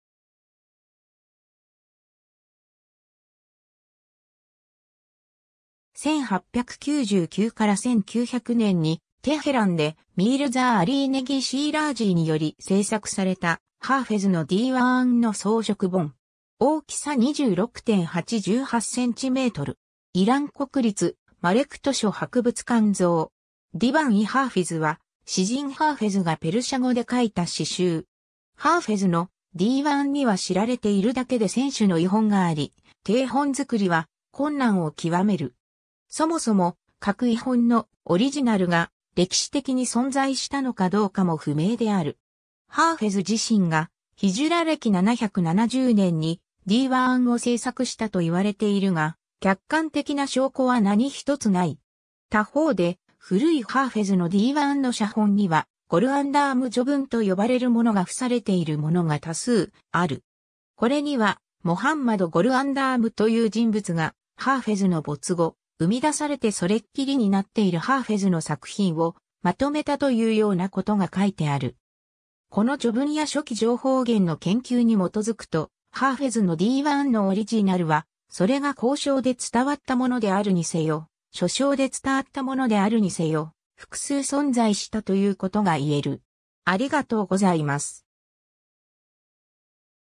ディヴァン・イ・ハーフィズ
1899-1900年にテヘランでミールザー・アリーネギー・シーラーズィーにより制作されたハーフェズのディーワーン（詩集）の装飾本。
大きさ26.8×18cm。
イラン国立マレク図書博物館蔵。
 ディヴァン・イ・ハーフィズ（ペルシア語: دیوان حافظ‎,、英語: The Divān of Hafez）は詩人ハーフェズがペルシャ語で書いた詩集。
ハーフェズの「ディーワーン」（詩集）には知られているだけで1000種の異本があり、底本作りは困難を極める。
そもそも各異本の「オリジナル」が歴史的に存在したのかどうかも不明である。
ハーフェズ自身がヒジュラ暦770年（西暦1368年）にディーワーンを制作したと言われているが、客観的な証拠は何一つない。
他方で、古いハーフェズのディーワーンの写本には「ゴルアンダーム序文」と呼ばれるものが付されているものが多数ある。
これには、モハンマド・ゴルアンダーム（Moḥammad Golandām）という人物がハーフェズの没後、産み出されてそれっきりになっているハーフェズの作品をまとめたというようなことが書いてある。
この序文や初期情報源（後述）の研究に基づくと、ハーフェズのディーワーンのオリジナルは、それが口承で伝わったものであるにせよ、書承で伝わったものであるにせよ、複数存在した（これらディーワーン群を以下、便宜的に「オリジナル」と呼ぶ）ということが言える。